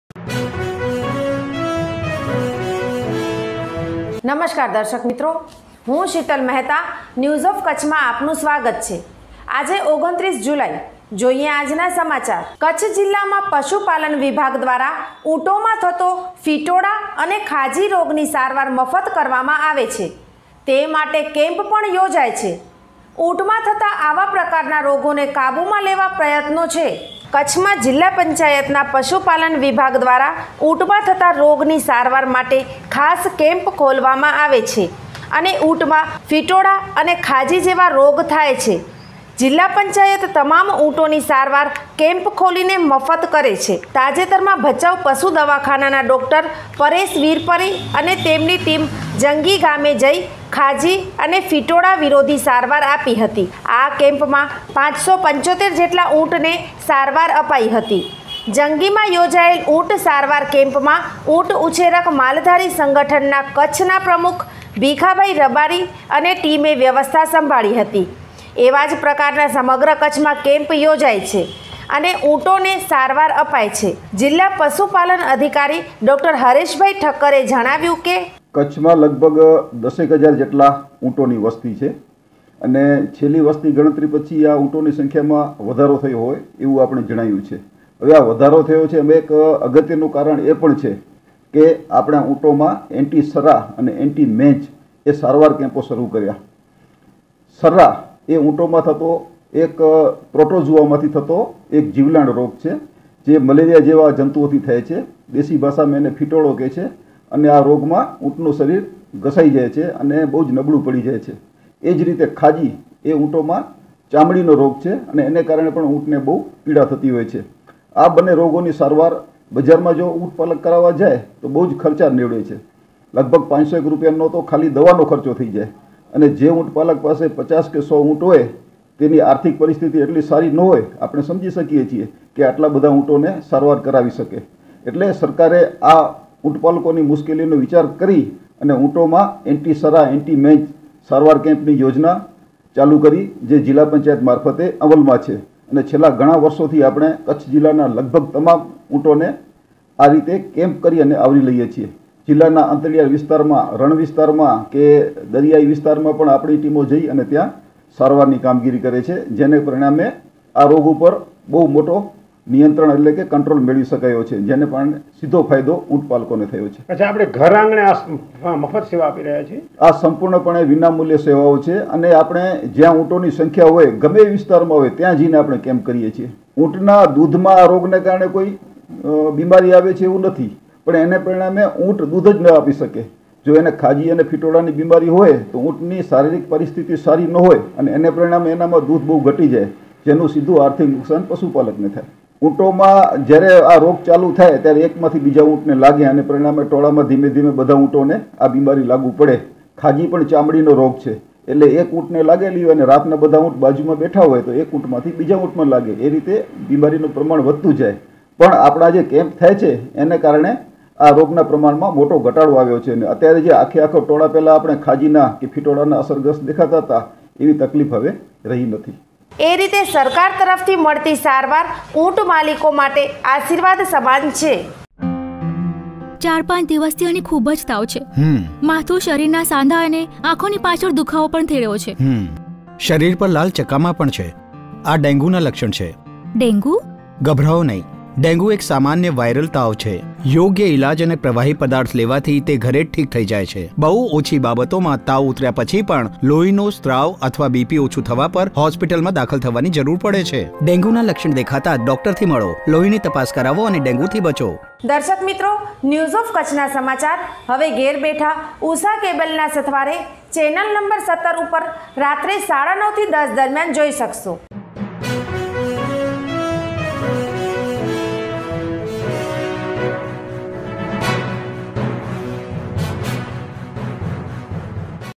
ઊંટમાં થતો ફિટોડિ અને ખાજીના રોગની સારવાર જિલ્લા પંચાયત દ્વારા મફત કરવામાં આવે છે
ઊંટ માં થતો  ફિટોડિ અને ખાજી ના રોગ ની સારવાર જિલ્લા પંચાયત દ્વારા મફત કરવામાં આવે છે રોગને કાબૂમાં લેવાનો પ્રયત્ન